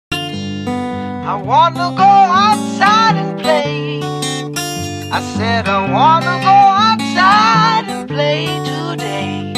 Yaad + Tukada 🐘🐘 Rare Asian Elephant Twins Having a Day Outside
Yaad + Tukada 🐘🐘 Rare Asian Elephant Twins

From what I've seen, if the twins realize they are apart from each other, it's not long before one runs over and finds the other!

Asian elephants are critically endangered, with an estimated 30,000 remaining in the wild, and one of their main threats is human-elephant conflict. The @syracusezoo is involved in several studies aimed at providing humans with better ways to coexist with wild elephants in their range countries.